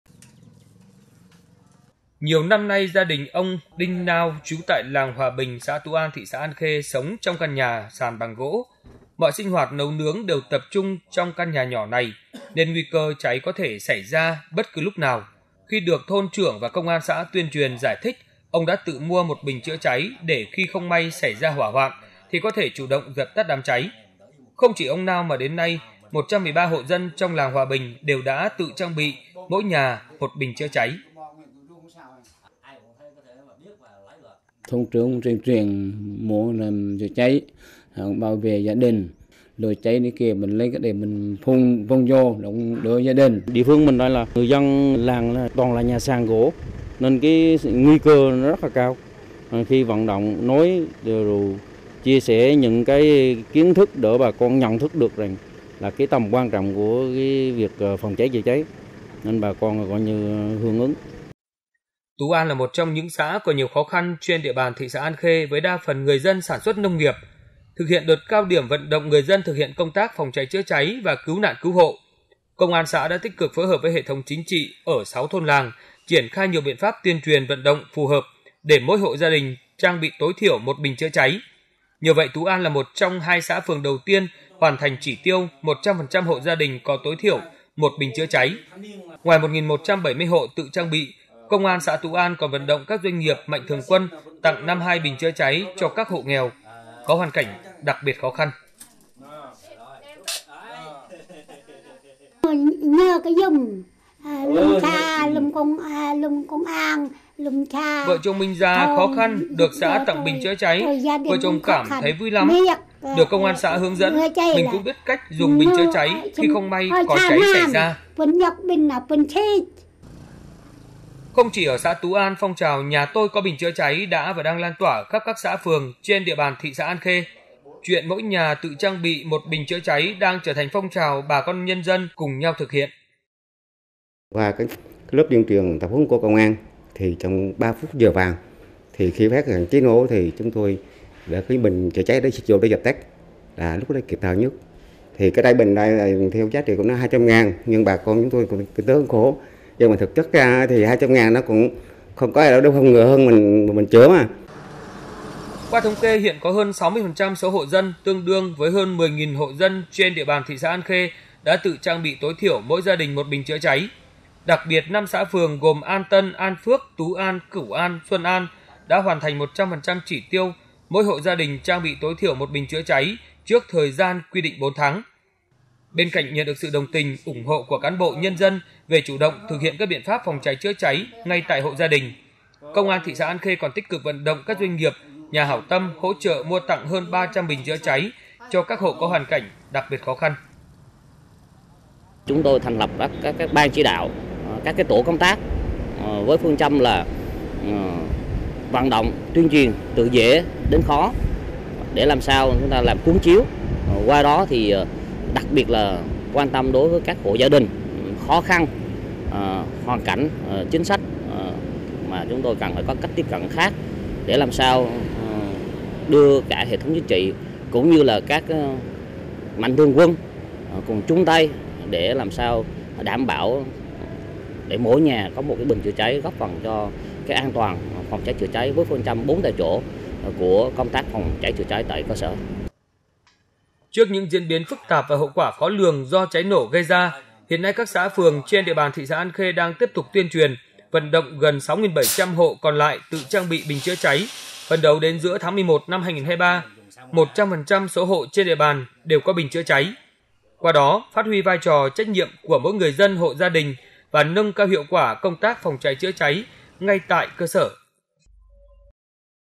Lan tỏa phong trào “Nhà tôi có bình chữa cháy” trong cộng đồng dân cư thị xã An Khê
Thực hiện Chỉ thị số 01 của Thủ tướng Chính phủ về “Tăng cường công tác phòng cháy, chữa cháy trong tình hình mới”, thời gian qua, Công an thị xã An Khê đã tích cực tham mưu cấp ủy, chính quyền địa phương triển khai đồng bộ nhiều biện pháp nhằm nâng cao hiệu quả công tác phòng cháy, chữa cháy trên địa bàn. Đến nay, 100%  hộ gia đình trên địa bàn có 1 người trong gia đình được tuyên truyền, tập huấn các kỹ năng phòng cháy chữa cháy, cứu nạn cứu hộ. Đặc biệt với phương châm “Hãy tự cứu mình trước khi được cứu”, phong trào “Nhà tôi có bình chữa cháy” ngày càng lan tỏa sâu rộng trong cộng đồng dân cư trên địa bàn thị xã An Khê.